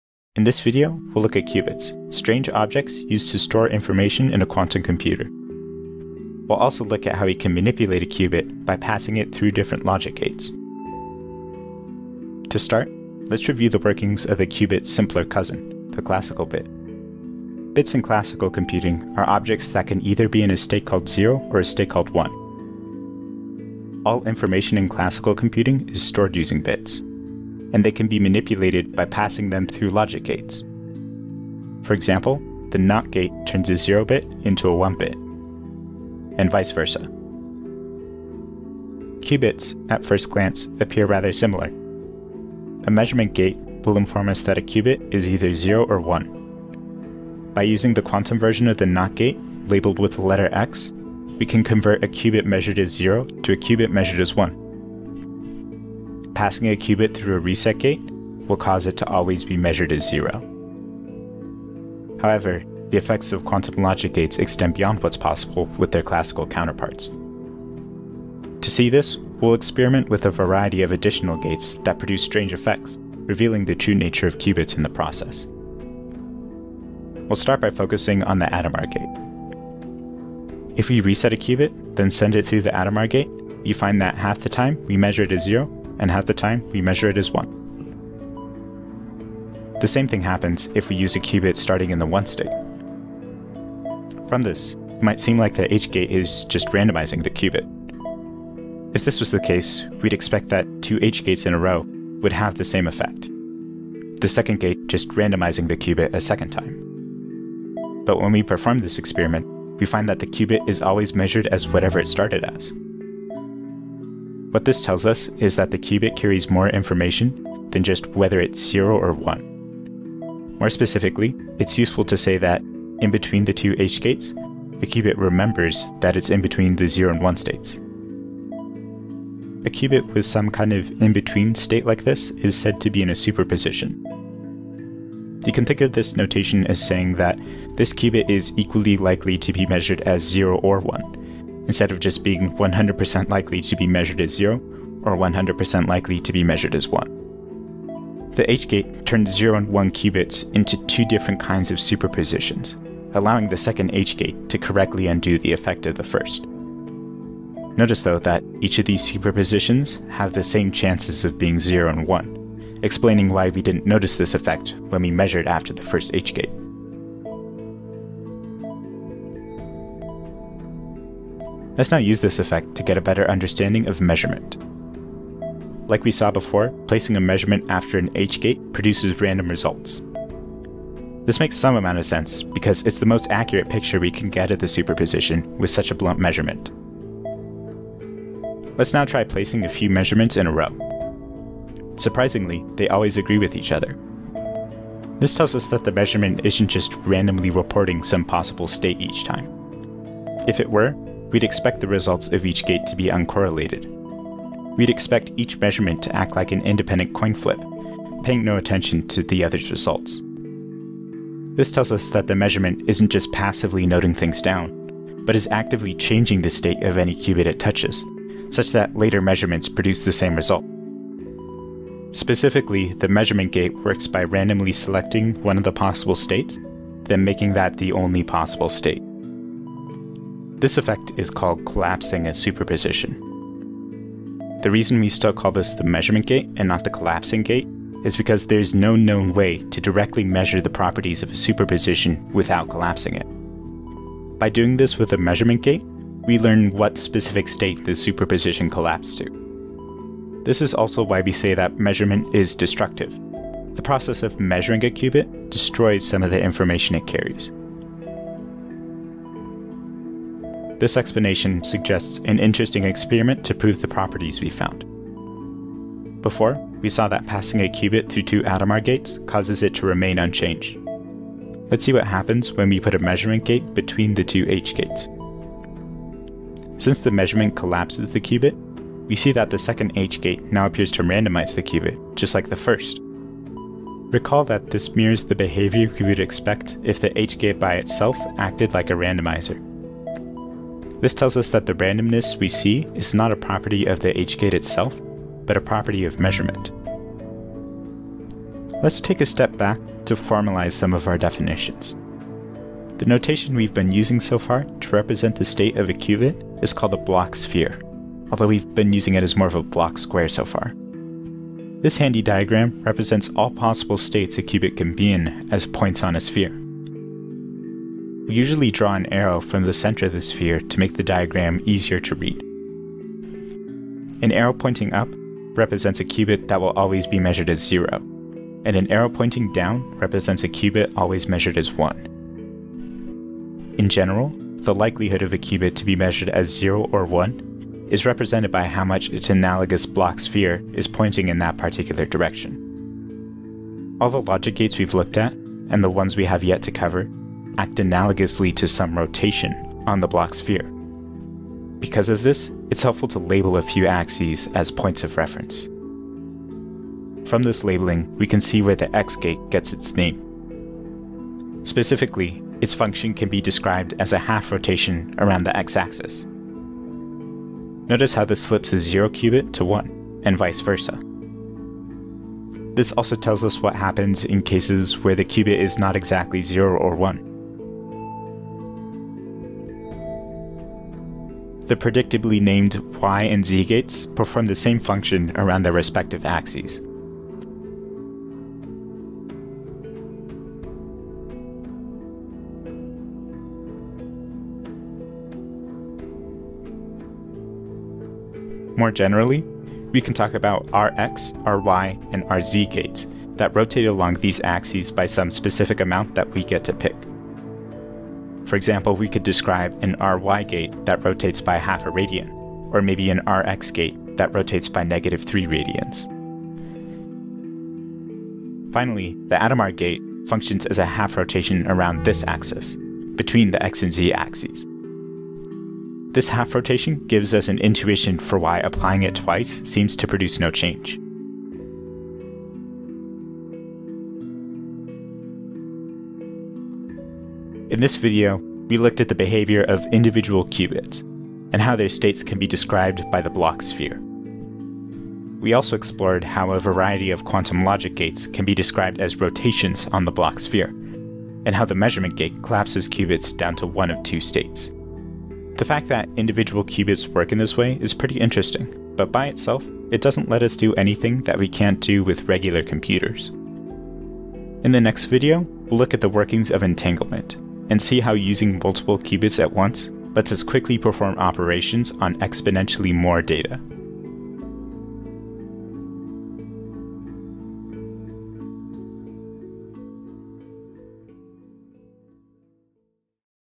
Logic Gates Rotate Qubits
In which we explore the strange effects of quantum logic gates, and what that tells us about the nature of qubits.

0:00 Intro
0:16 Classical Bits
0:38 Similar Qubit Behavior
1:20 The Hadamard Gate
2:56 Measurement
4:25 Measuring Between H Gates
5:02 The Bloch Sphere
5:43 Logic Gates As Rotations
7:16 Review